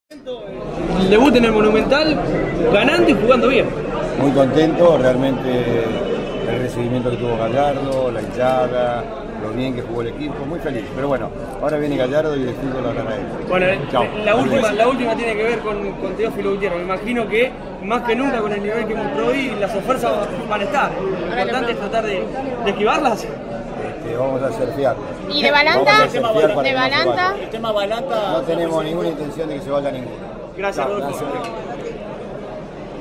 D´onofrio post River 2 Central 0 Metegol
La palabra del presidente de River Plate post triunfo 2 - 0 sobre Rosario Central